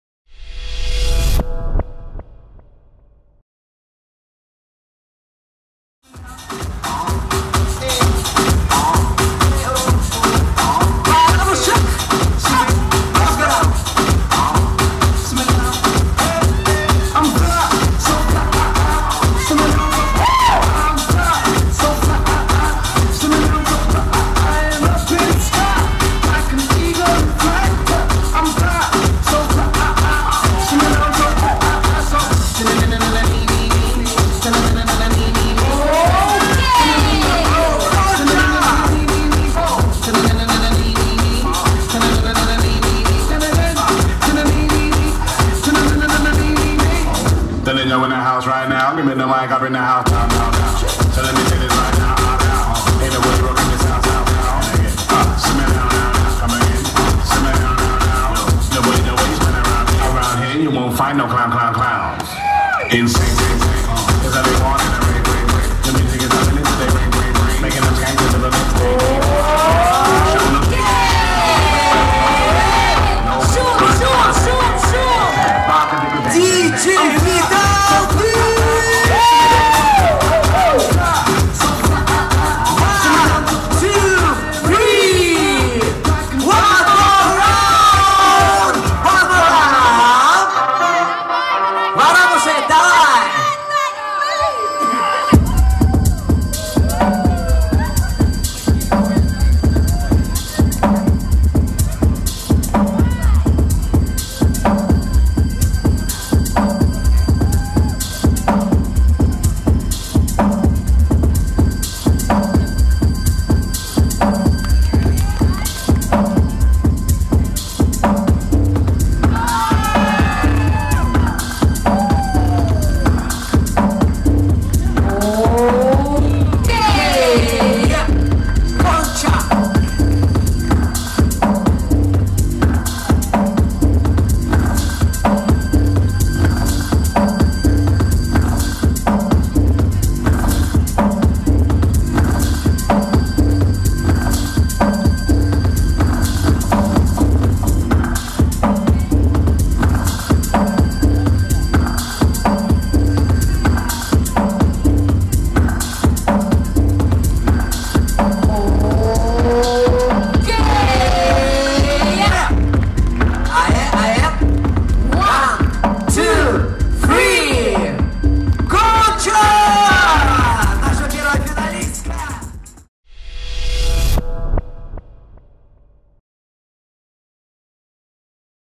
RESPECT MY TALENT-2014. Beginners House 1/2 Gonca Karakas vs Vorobushek
RESPECT MY TALENT-2014 dance event.
26-27th of September, St.Petersburg, Russia.
HOZIN, MAMSON, NIAKO, GRICHKA, CALEAF, PARADOX, DJ's KONICHIWA CREW (AFRO-KID & VIDAL BEATZ).

Org. by Olya & Polya